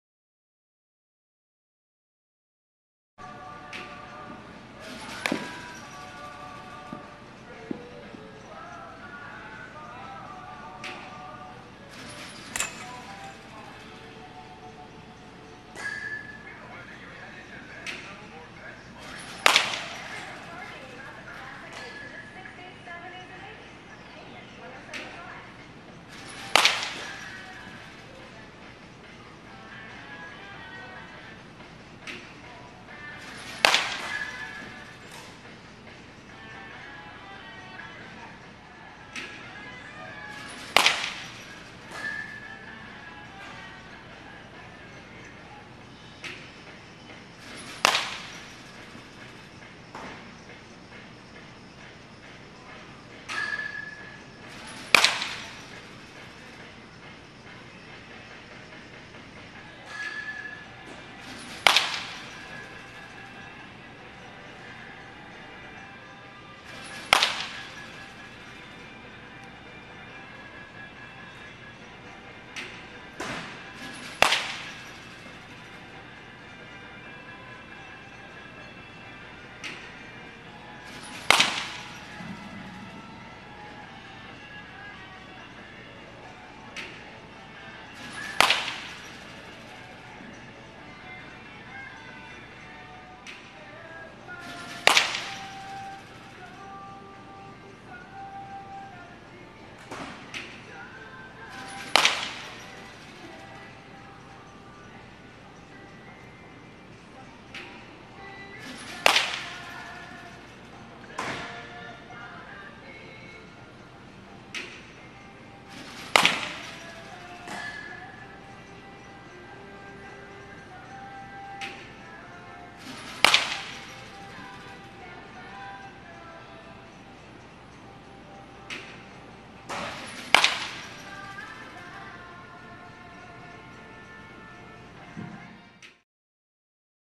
2011 Fall Ball Working on OUTSIDE Pitches
Working on outside pitches simulating a 2 strike approach using Lousiville Slugger 33in 30oz Maple bat.